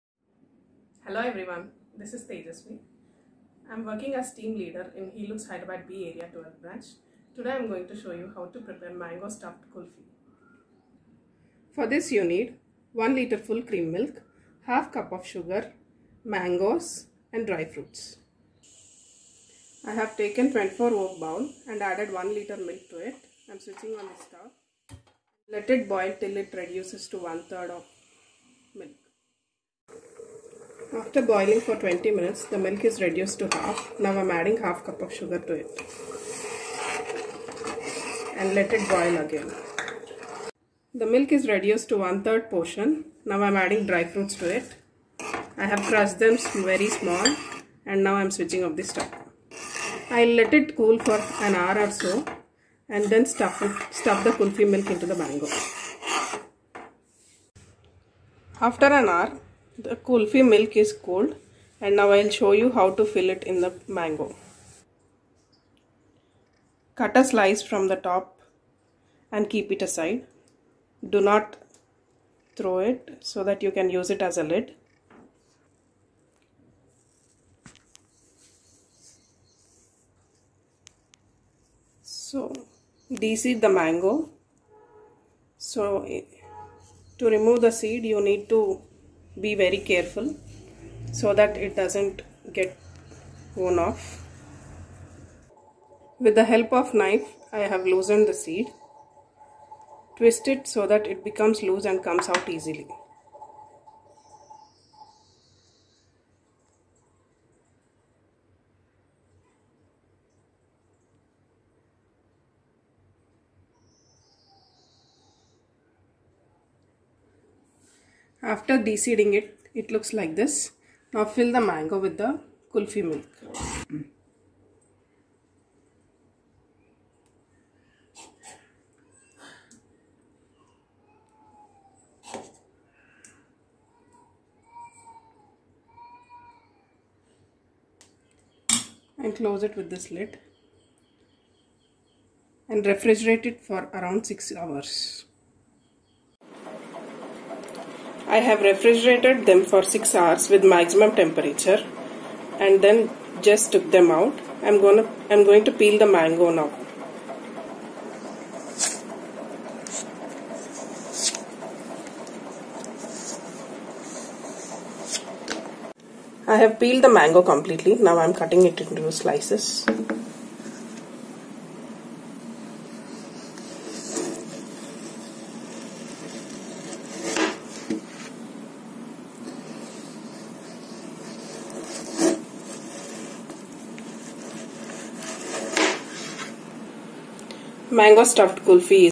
Mango Stuffed Kulfi Cooked in Healux Cookware | Healux Good Food Club
Mango Stuffed Kulfi Cooked by Chef. Tejasvi from Hyderabad 12  Branch

Healux Cookware